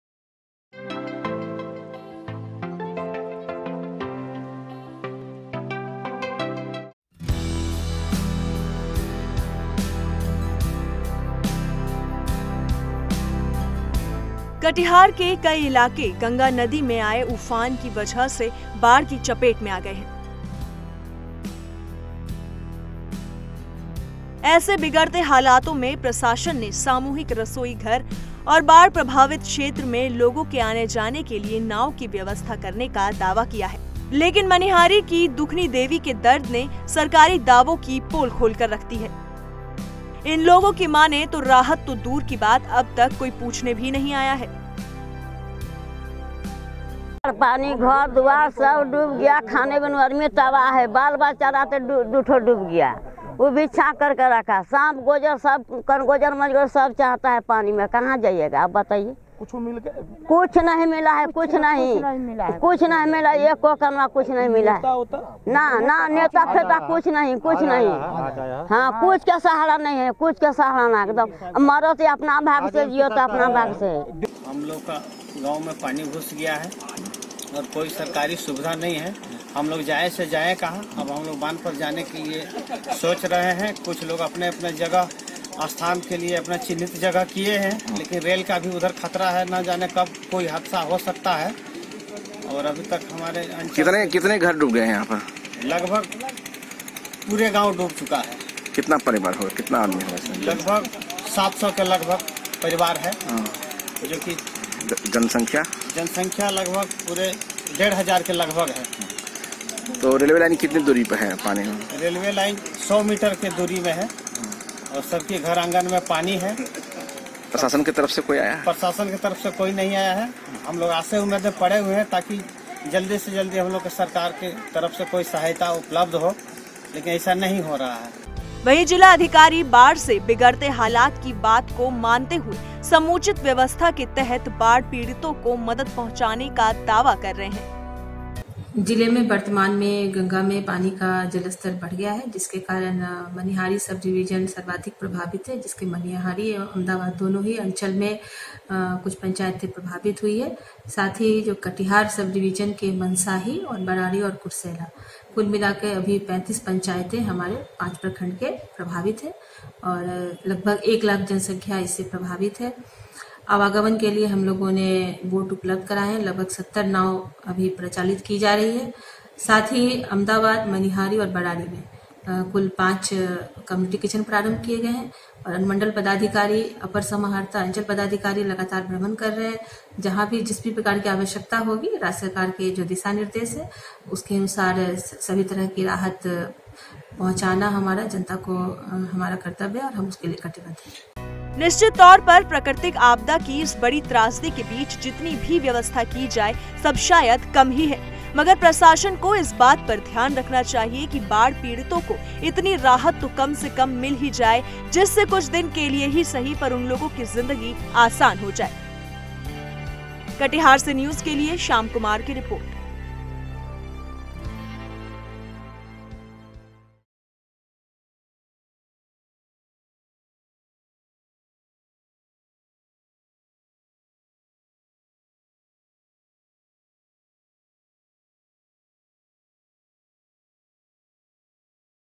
जानिए Katihar में आई बाढ़ में प्रशासन के दावों का सच
कटिहार के कई इलाके गंगा नदी में आए उफान की वजह से बाढ़ की चपेट आ गए हैं...ऐसे बिगड़ते हालातों में प्रसाशन ने सामूहिक रसोई घर और बाढ़ प्रभावित क्षेत्र में लोगों के आने जाने के लिए नाव की व्यवस्था करने का दावा किया है...